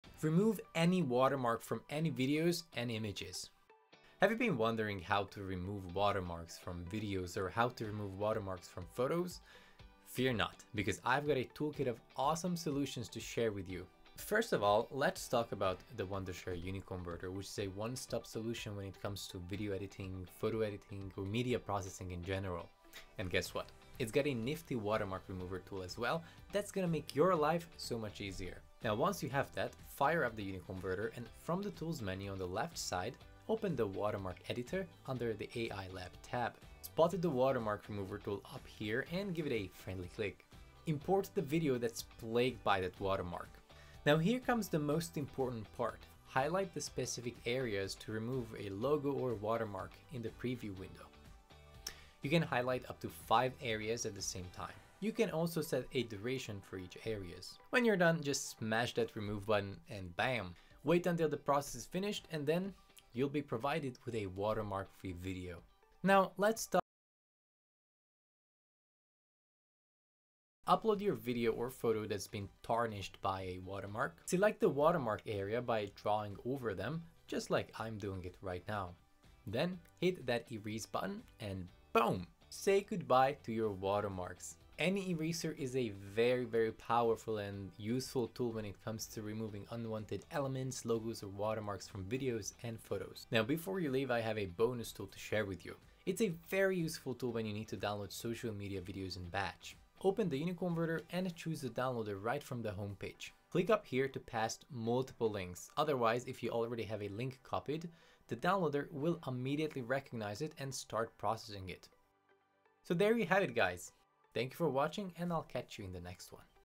How to Remove Watermark from Photos and Videos
Learn how to easily remove watermarks from your photos and videos in this helpful tutorial. Say goodbye to those pesky watermarks with these simple steps!

⌚ Timestamps
0:00 - Intro
0:42 - Remove watermark from photo
1:08 - Remove watermark from video
1:47 - Wrap up